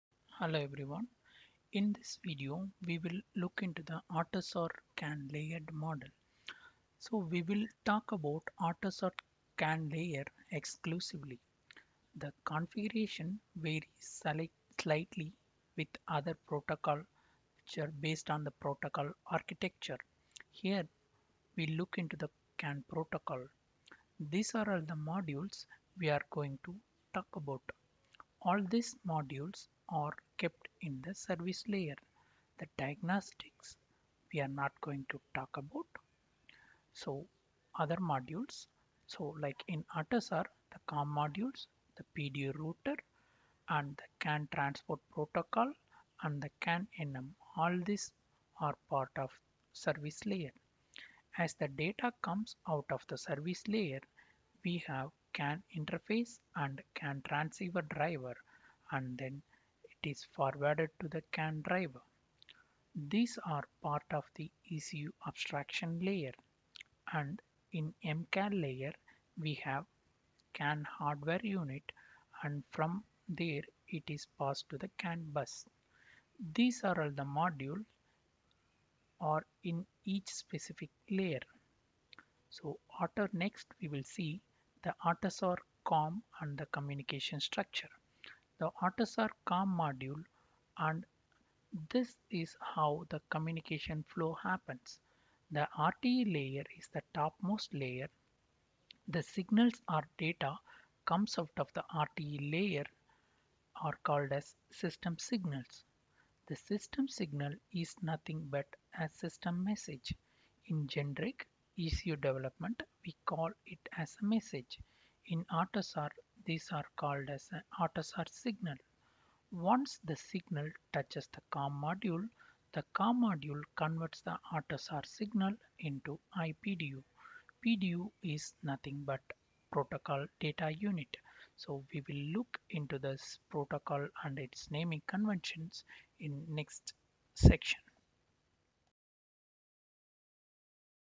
AUTOSAR CAN Layer model and CAN communication Structures of COM module
This video talk about AUTOSAR COM module CAN layer model and Communication structure followed in AUTOSAR. Answers the questions of how messages transferred from COM.